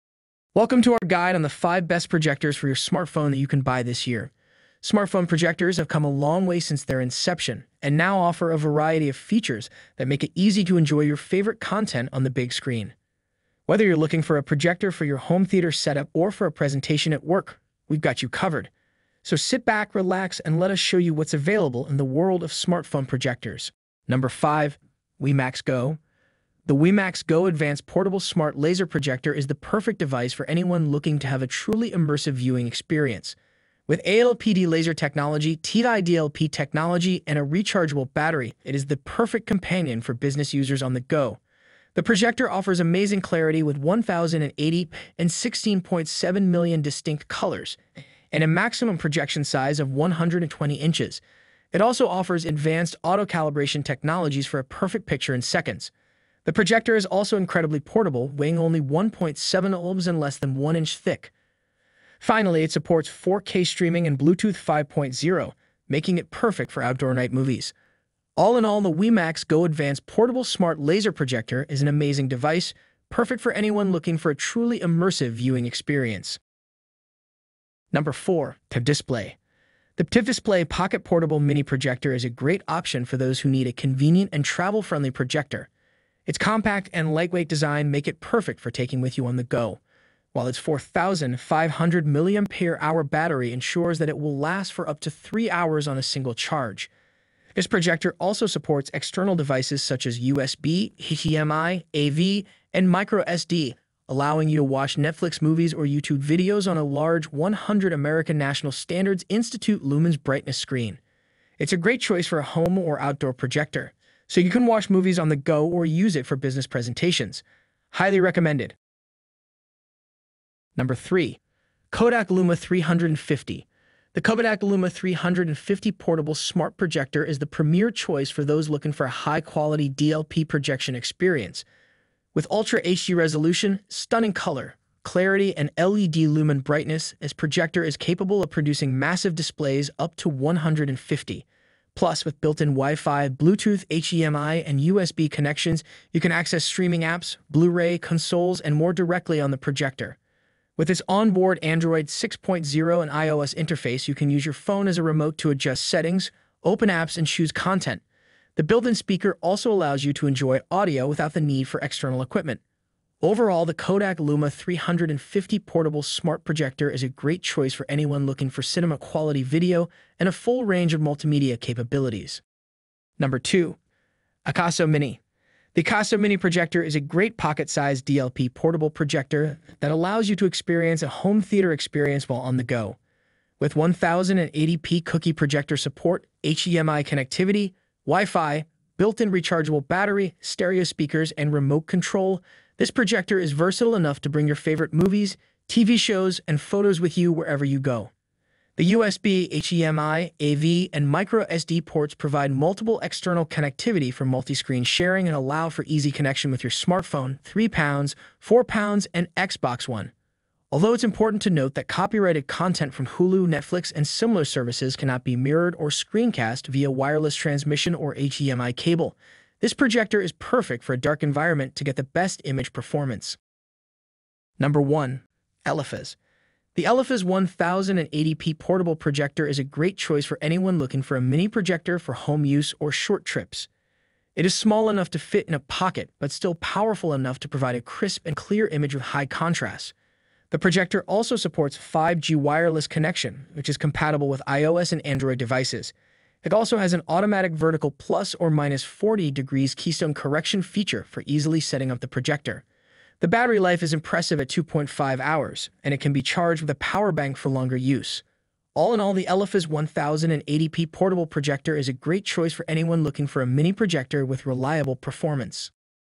5 Best Projectors For your Smartphone | Mini Projector in 2024
Here are The Products to Buy
5 ► WEMAX Go
Amazon US

4 ► PTVDISPLAY
Amazon US

3 ► KODAK Luma 350
Amazon US

2 ► AKASO Mini
Amazon US

1 ► ELEPHAS
Amazon US